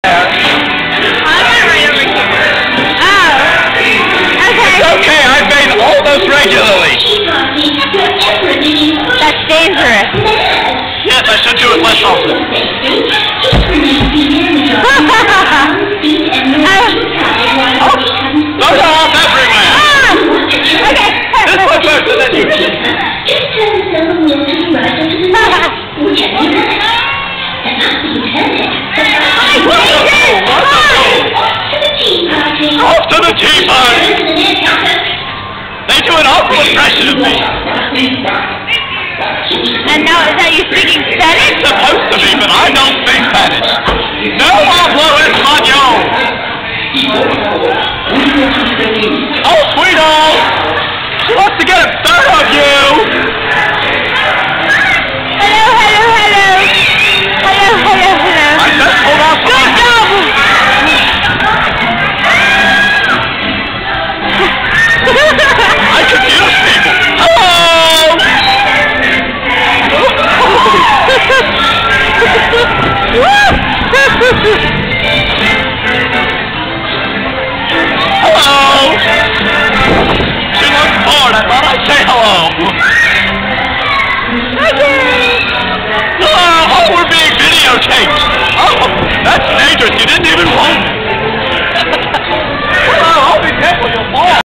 Mickey's Very Merry Christmas Party 2008 Mad Hatter
November 24th, 2008 at around 11:45pm, just before the party was over, after riding the tea cups twice in a row since no body was in line, we were just about ot leave when the Mad Hatter and the Tweedles came in...and I got to ride with the Mad Hatter!